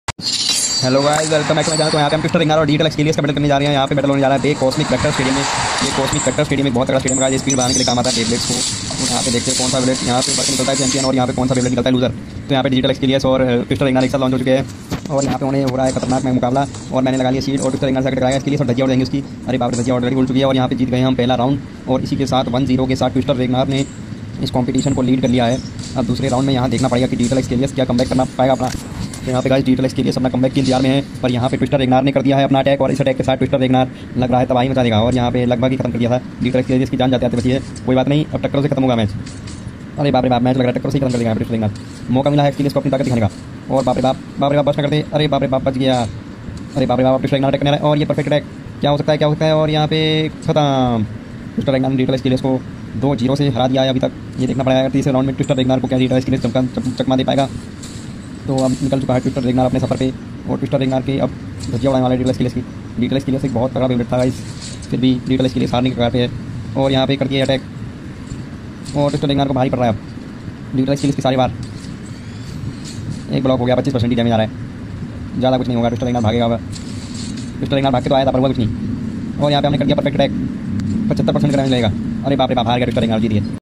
Twister Regnar vs Digital Xcalius|Beyblade Burst Quadstrike
Hello Guys,
In this video we will see battle between best beyblade burst Quadstrike beys.